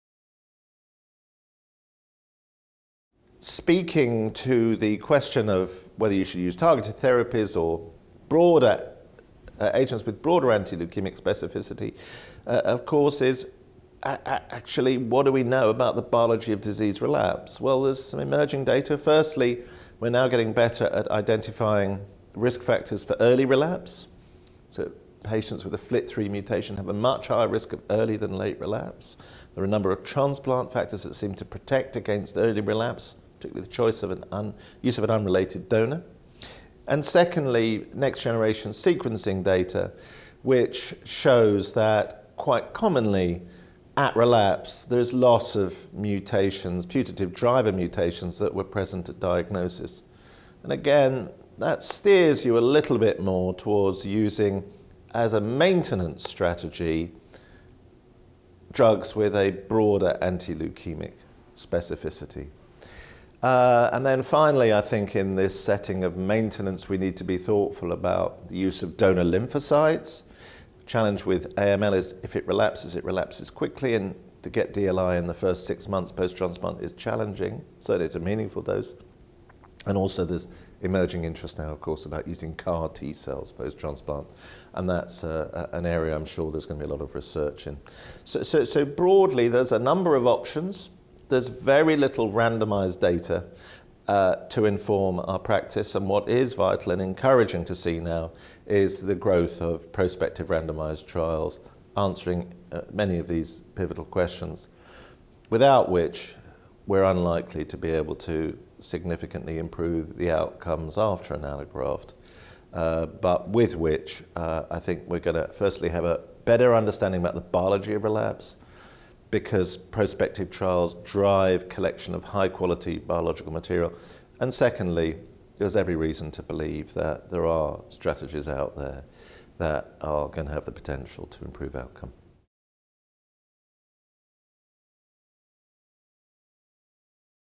Targeted vs. broader agents for maintenance in AML
Charles Craddock, CBE, FRCP (UK), FRCPath, DPhil, of the University of Birmingham, Birmingham, UK, discusses the use of targeted vs. broader agents in acute myeloid leukemia (AML). This interview was recorded at the 2018 European School of Hematology (ESH) Clinical Updates on Acute Leukemias, held in Budapest, Hungary.